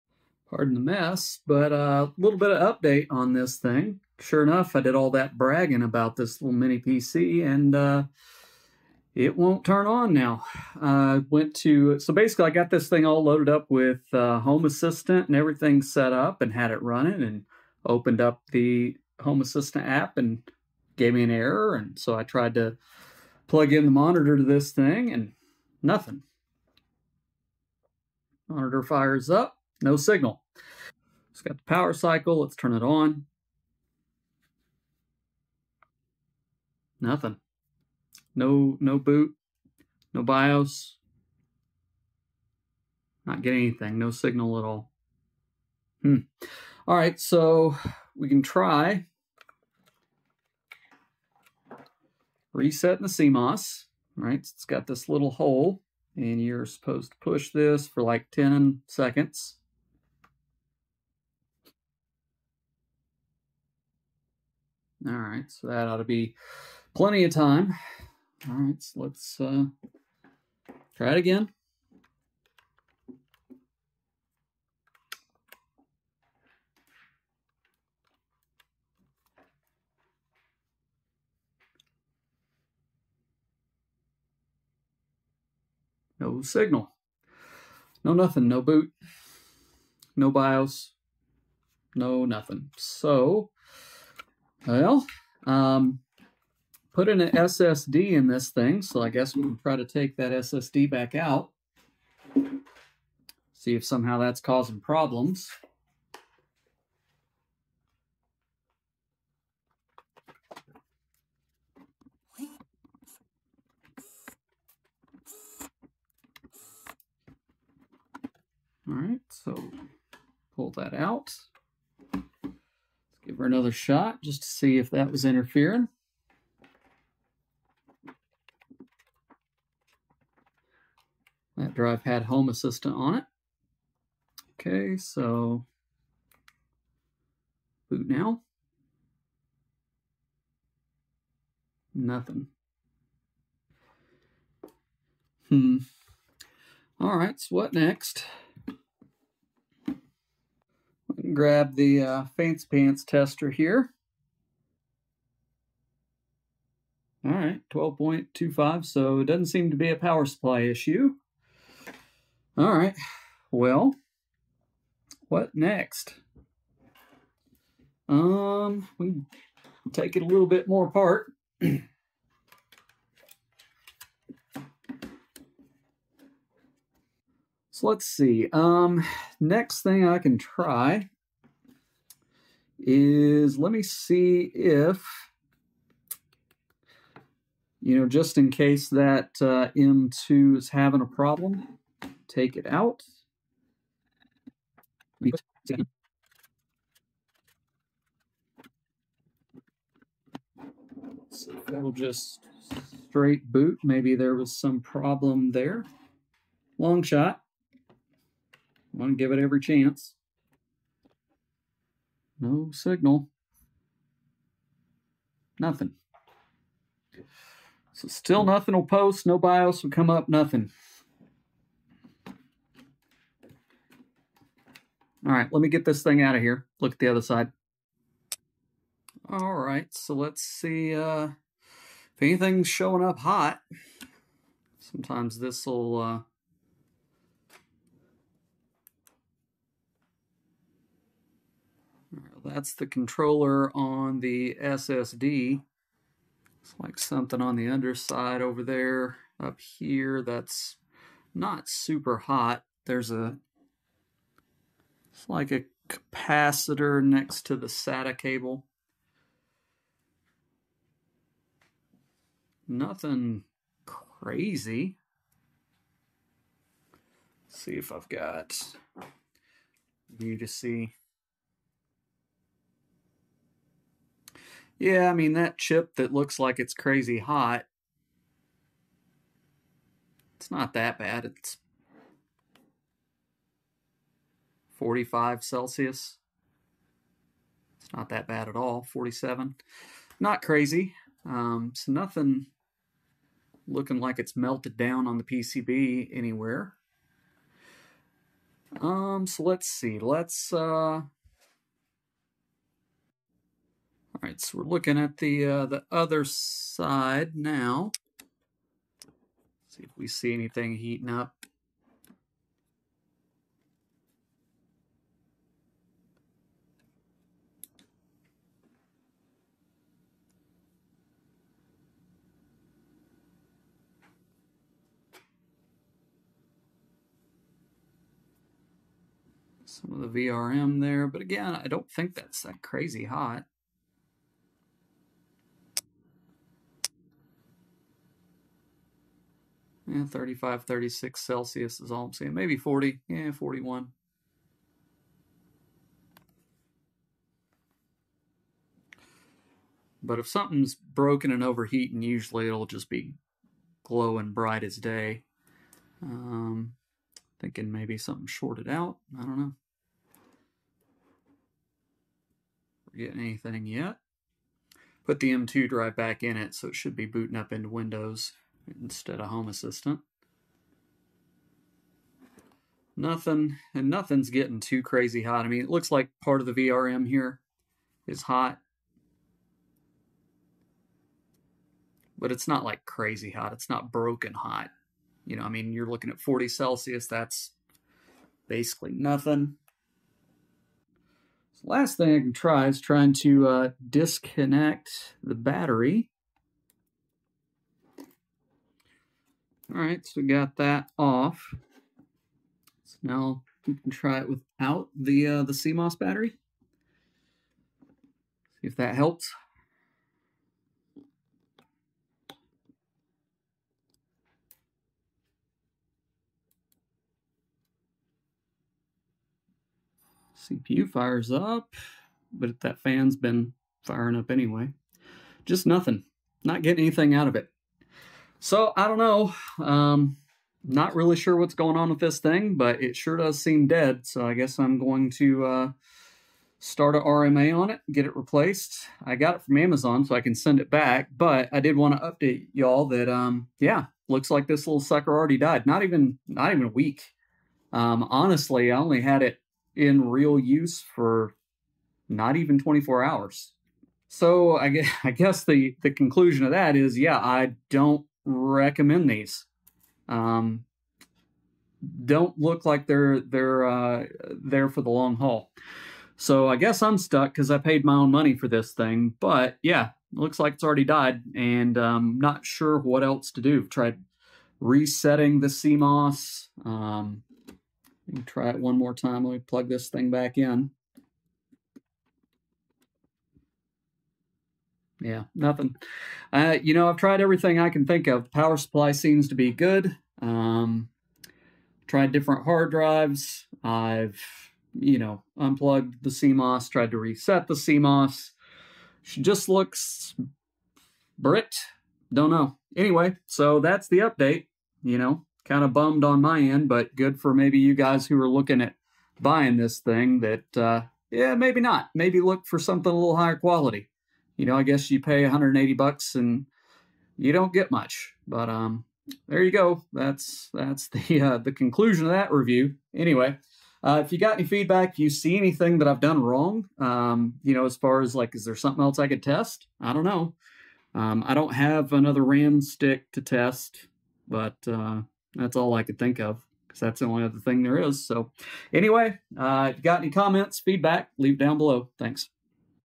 My Beelink Mini PC Died in 4 days
Well, I said I'd let you know if anything happened. After setting up home assistant, and getting everything dialed in... my Beelink S12 Pro died the very next day. In this video, I try to get it to boot to no avail... Oh well.

Caveat Emptor.

=== Timestamps ===
0:00 Introduction
0:30 Power Cycling
0:50 Resetting the CMOS
1:34 Removing new SSD
2:30 Voltage testing the power supply
2:58 Removing M2 SSD
3:45 Thermal imaging
6:30 Disconnecting CMOS battery
7:05 Conclusion: IDK WTF.